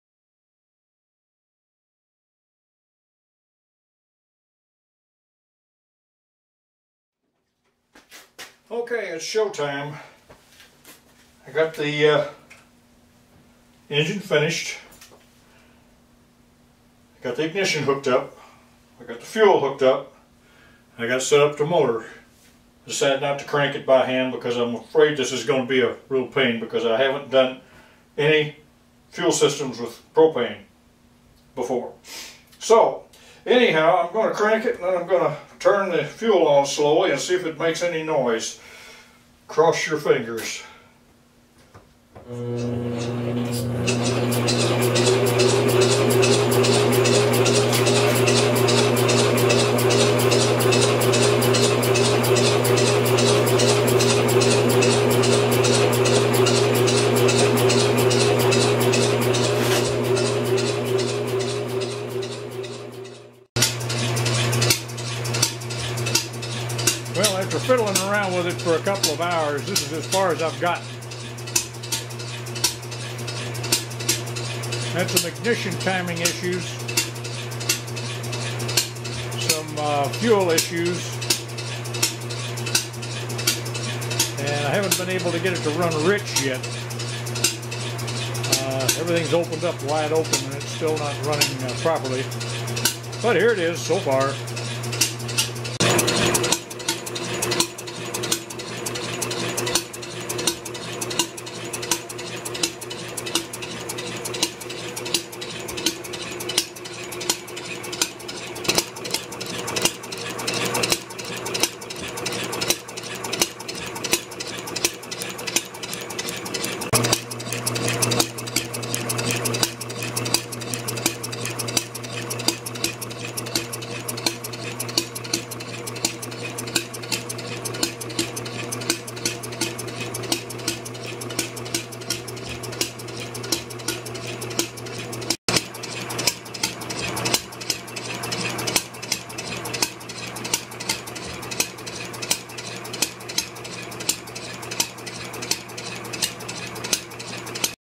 Upside Down Engine #1
This is the first test of the Upside Down Engine.  As usual, it doesn't want to wake-up but at least now I know what I need to do to get it to run.